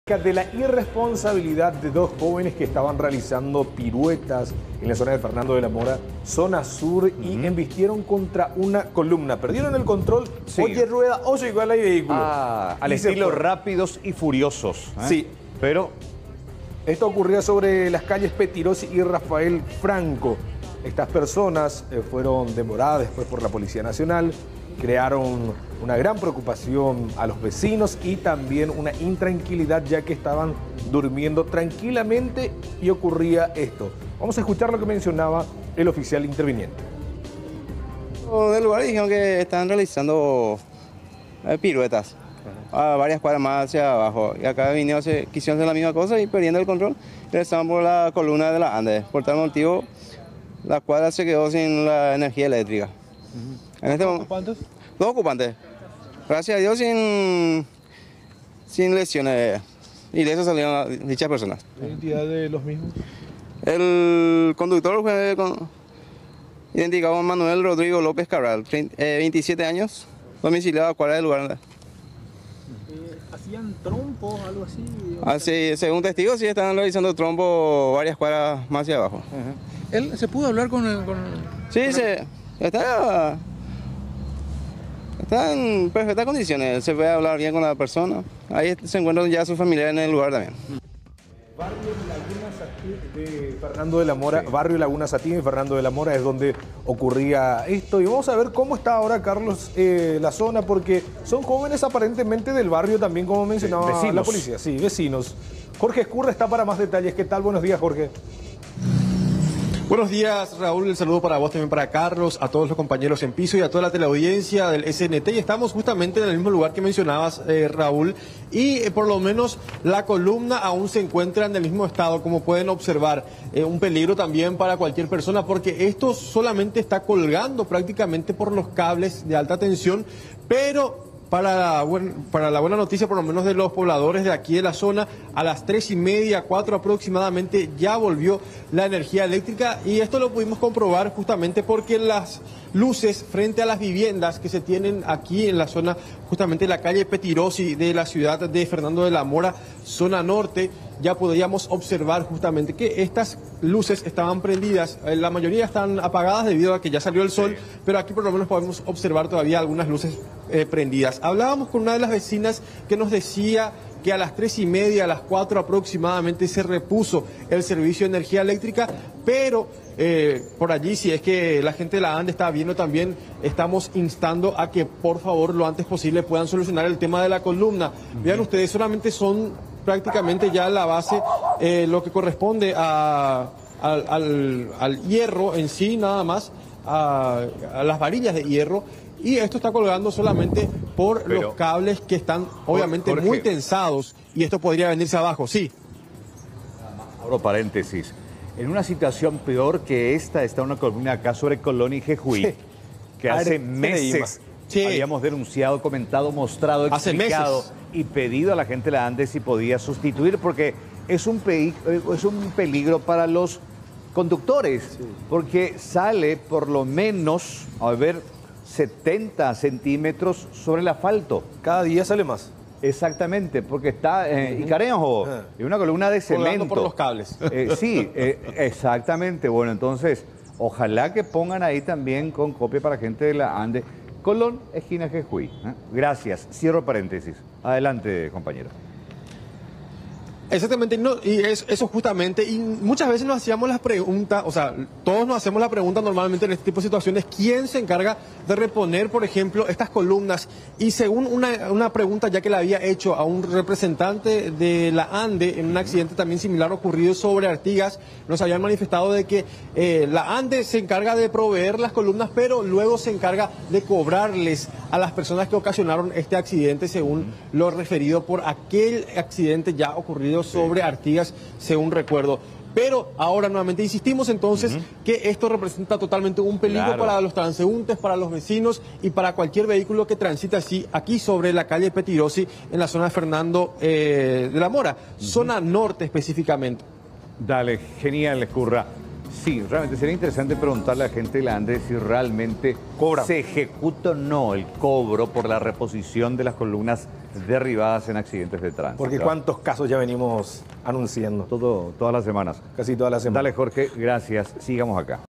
Dejan sin luz a un barrio completo tras chocar contra una columna
Los jóvenes realizaban piruetas en su automóvil y perdieron el control